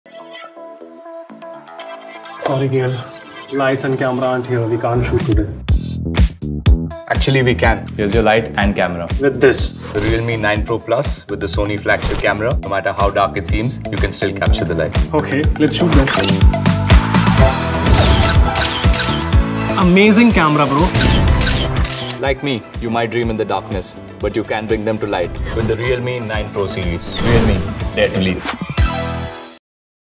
realme 9 Pro+ 5G | KL Rahul
No amount of darkness is too much when you can CaptureTheLight with the realme9Pro+ 5G’s Flagship Sony IMX766 OIS Camera.

Add light to your playground of dreams, just like our realme Captain KL Rahul did!